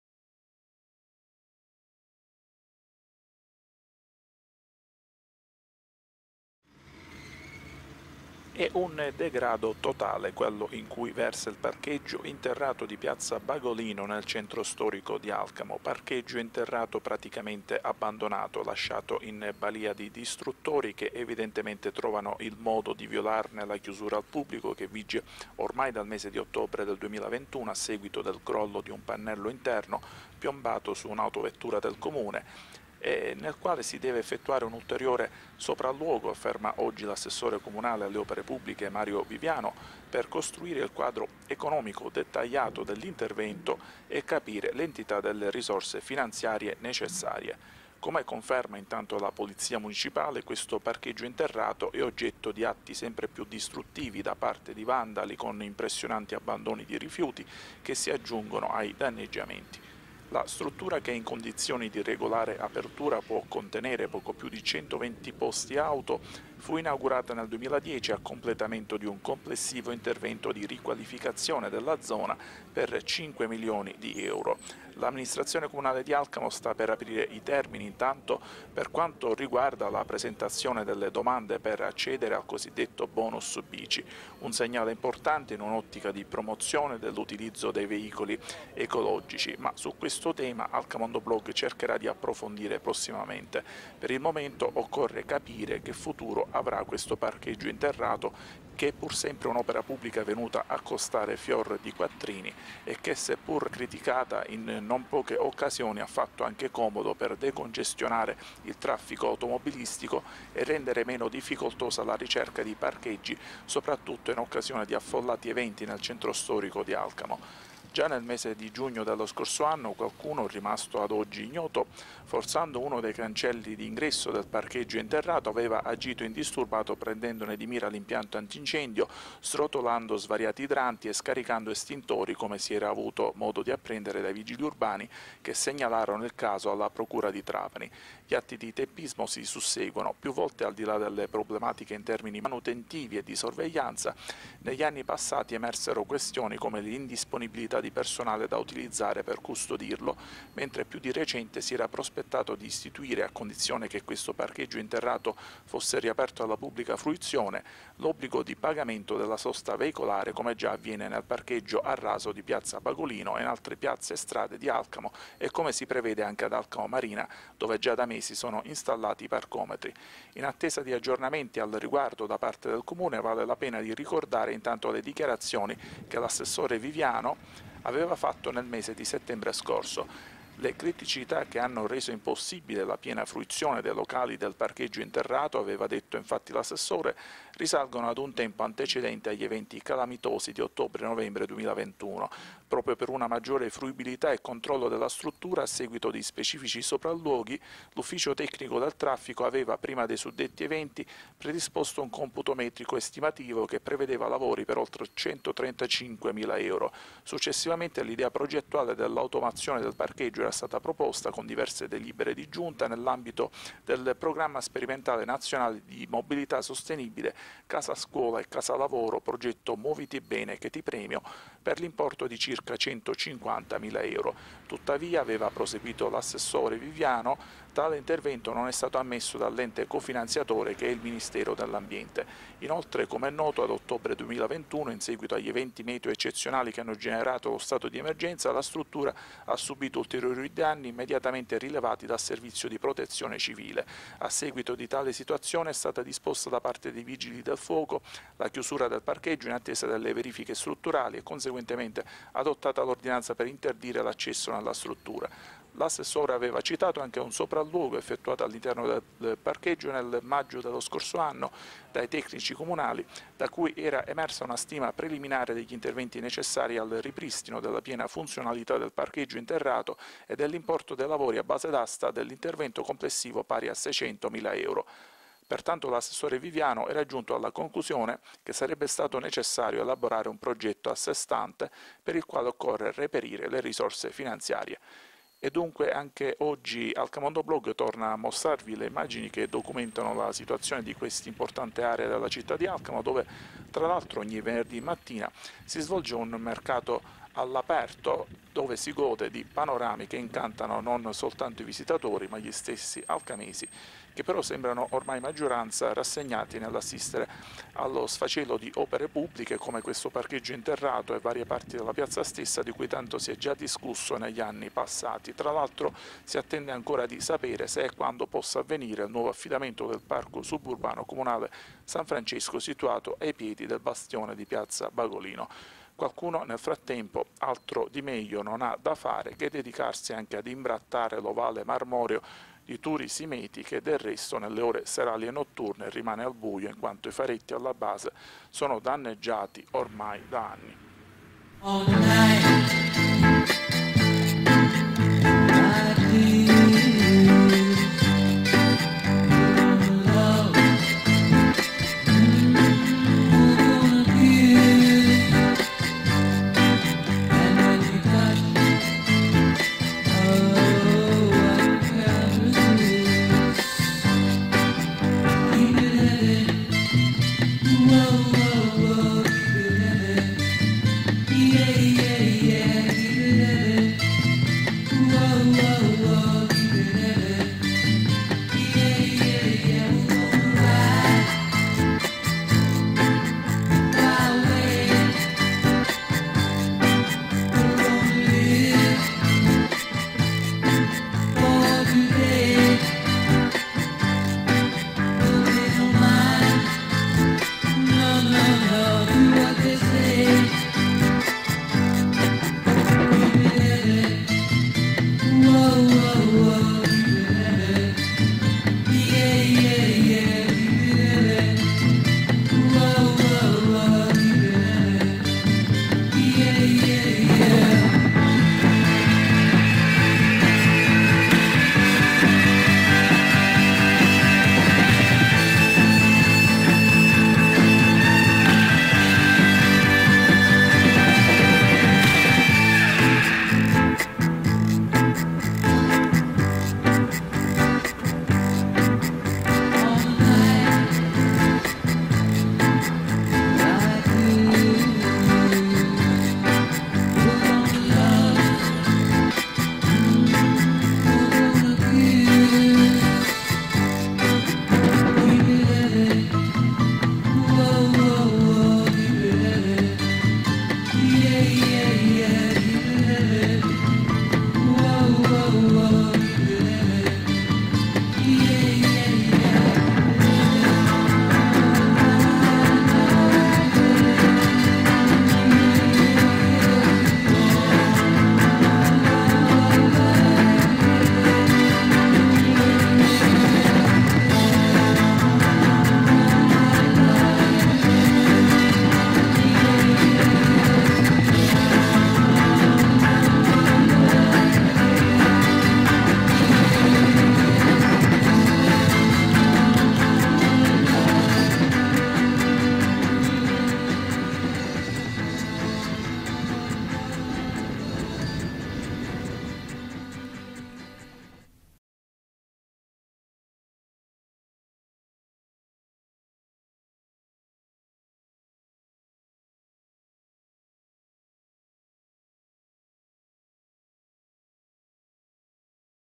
Alcamo, lo sfacelo del parcheggio interrato comunale chiuso da 17 mesi
Venerdì 17 marzo 2023 - E’ un degrado totale, quello in cui versa il parcheggio interrato di piazza Bagolino nel centro storico di Alcamo. Parcheggio interrato praticamente abbandonato, lasciato in balìa di ignoti distruttori, che evidentemente trovano il modo di violarne la chiusura al pubblico che vige ormai dal mese di ottobre del 2021 a seguito del crollo di un pannello interno, piombato su un’autovettura del Comune, e nel quale “si deve effettuare un ulteriore sopralluogo – afferma oggi l’assessore comunale alle opere pubbliche Mario Viviano - per costruire il quadro economico dettagliato dell’intervento e capire l’entità delle risorse finanziarie necessarie” per avviare la manutenzione.
Come conferma intanto la Polizia municipale, questo parcheggio interrato è oggetto di atti sempre più distruttivi da parte di vandali, con impressionanti abbandoni di rifiuti che si aggiungono ai danneggiamenti.
Tra l’altro, si attende ancora di sapere se e quando possa avvenire il nuovo affidamento del Parco suburbano comunale “San Francesco”, situato ai piedi del bastione di piazza Bagolino. Qualcuno, nel frattempo, altro di meglio non ha da fare che dedicarsi anche ad imbrattare l’ovale marmoreo di Turi Simeti che, del resto, nelle ore serali e notturne rimane al buio in quanto i faretti alla base sono danneggiati ormai da anni.